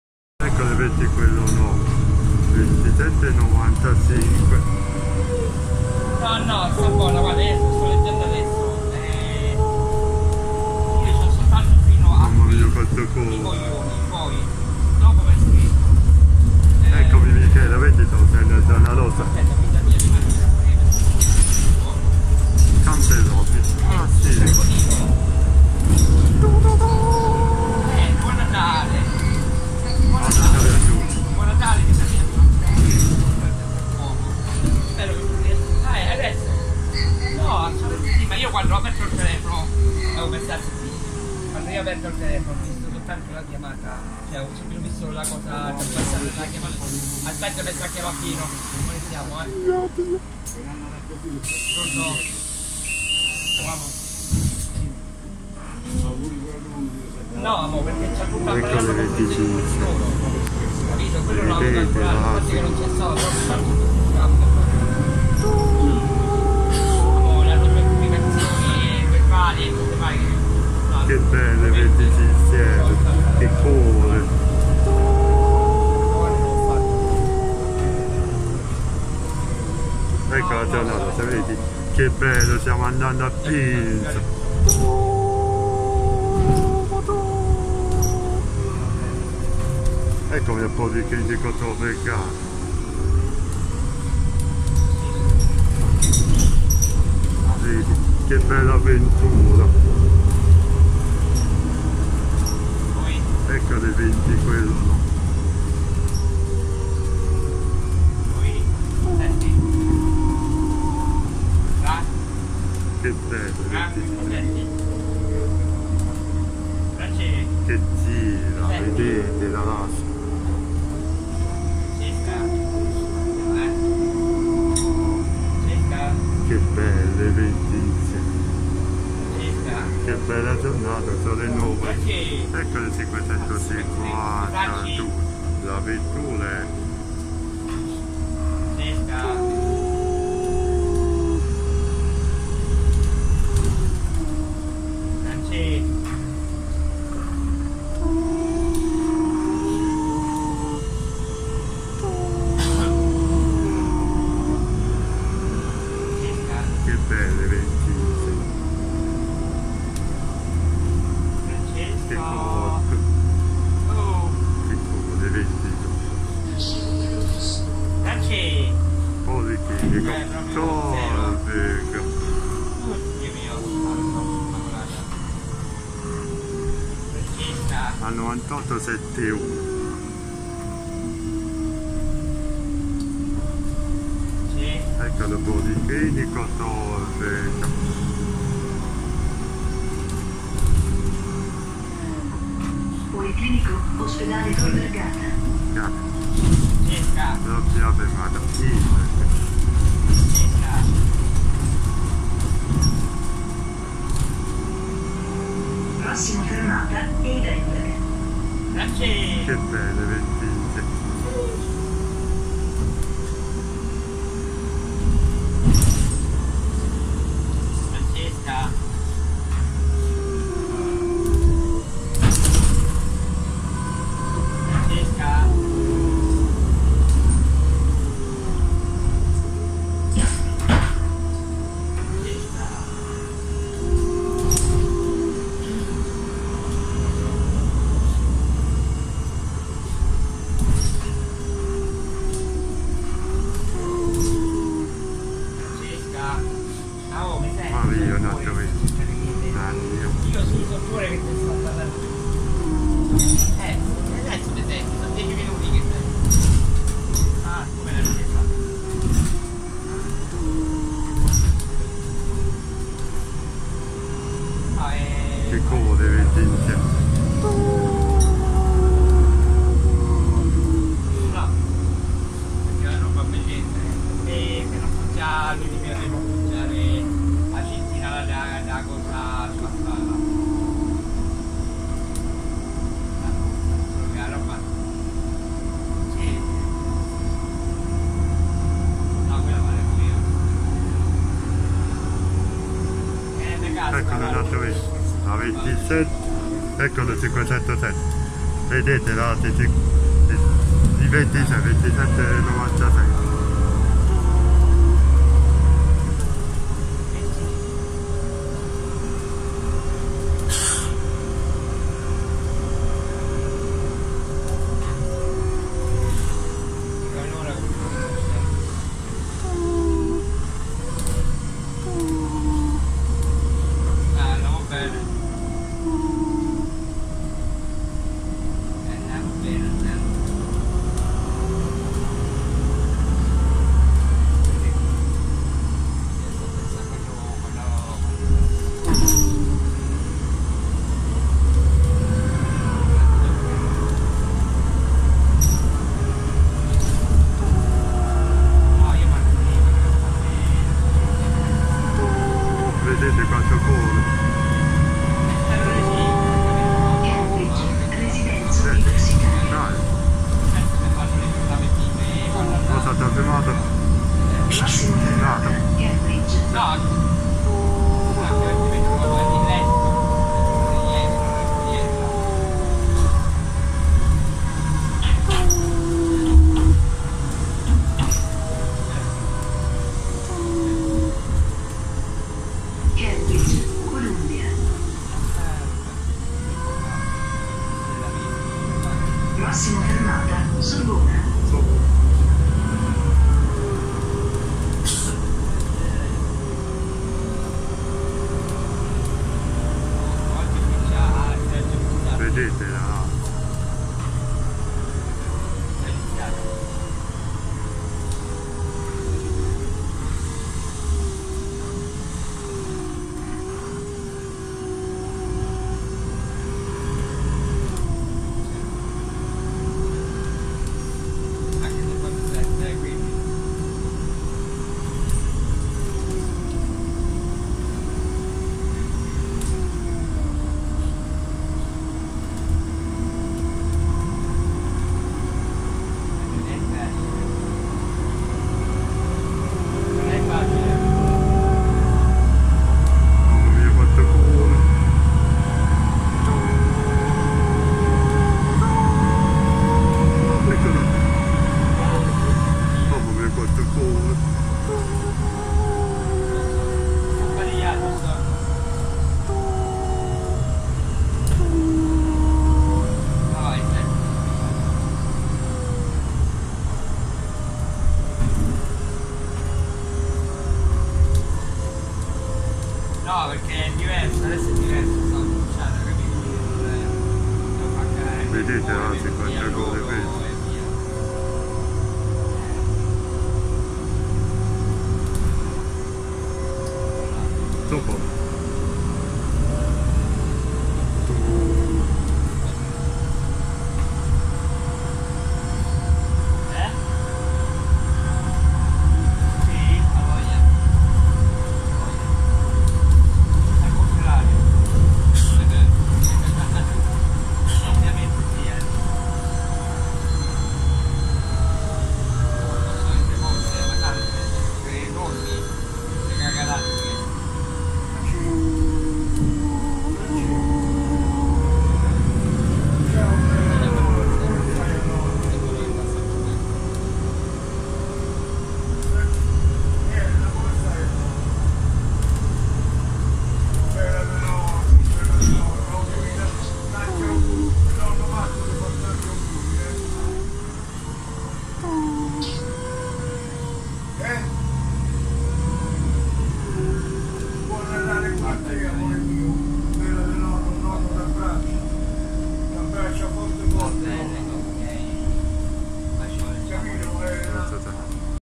Linea 20 direz. Anagnina la vettura è 2795 che è vuoto che corre autobus nuovo rosso
Linea 507 direzione grotte celoni la vettura è 2671 linea 20 direz. Cambellotti la vettura è 2796 linea 552 direz. Gerani la vettura è 9871 la zona rossa il viaggio